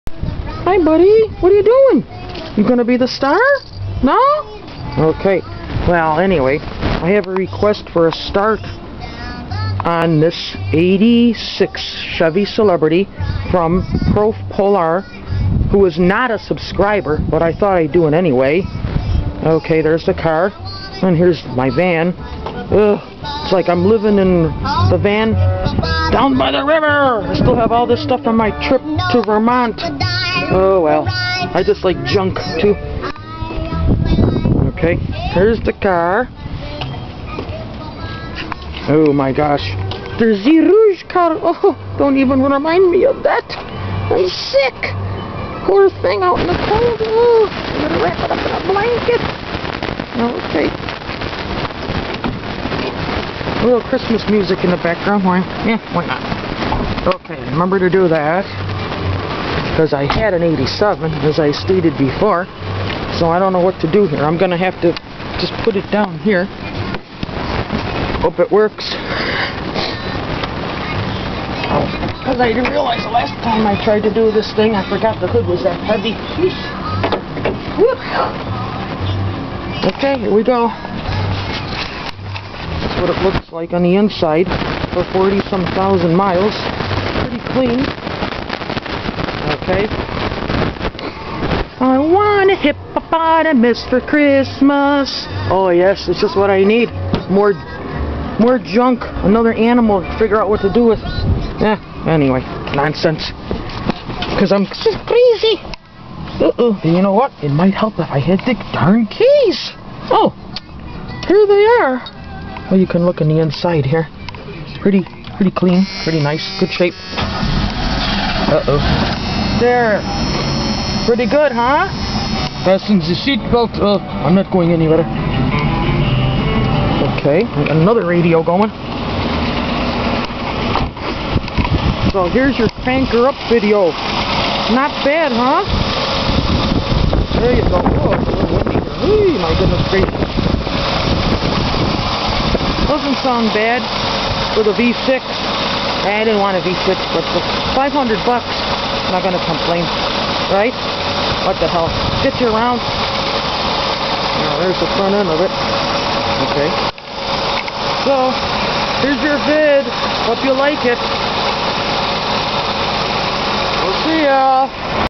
ZE CRANKER-UP!!!! ZE CELEBRITY!!!!
Starting ze '86 Celebrity for PROFPOLAR...